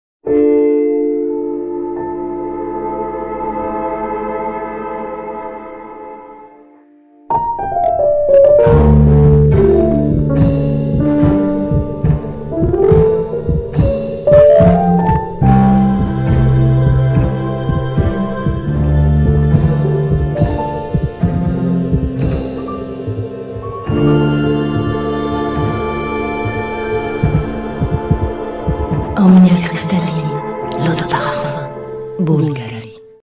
Omnia Crystalline EDP - Bvlgari
Omnia Crystalline é uma joia cintilante de luz, iluminando e refletindo a sensualidade suave e luminosa feminilidade da mulher. Tão delicado como uma flor requintada, um perfume floral amadeirado radiante.